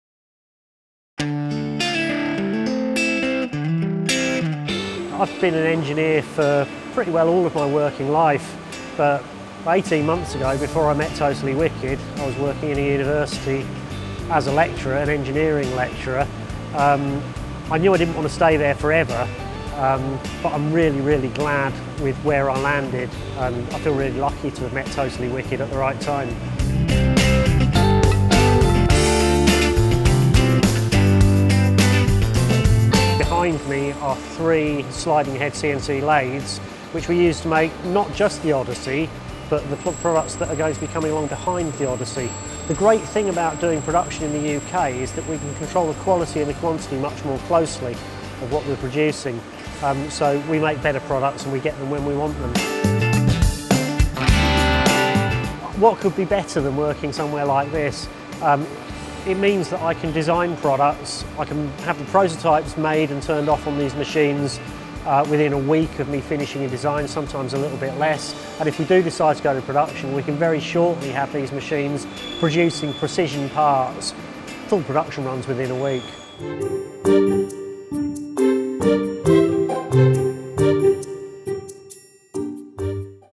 Totally Wicked E-cig Engineering
Totally Wicked Engineering Ltd was founded in 2013 to provide Totally Wicked e-liquid with the ability to design, engineer and manufacture a unique range of electronic cigarette products in the UK. The fundamental principles required to do this to our satisfaction are elegant and effective design, precision machining and manufacture, and high quality assembly and inspection. Making the best vaping “toys” is something we take extremely seriously.